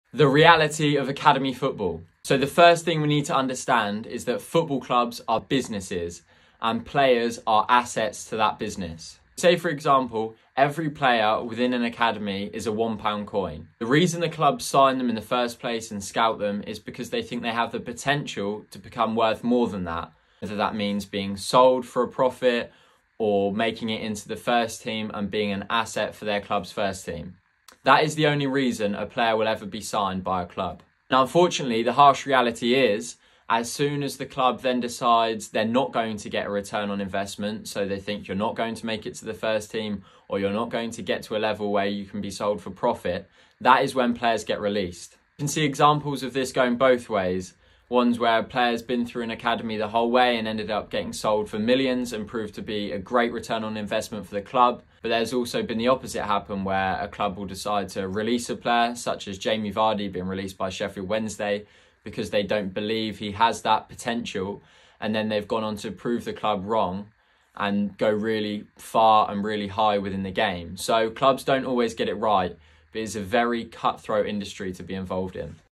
The reality of academy football
A breakdown into how football clubs, as businesses, actually operate.

This is unfortunately a reflection of how much money is pumped into football clubs nowadays.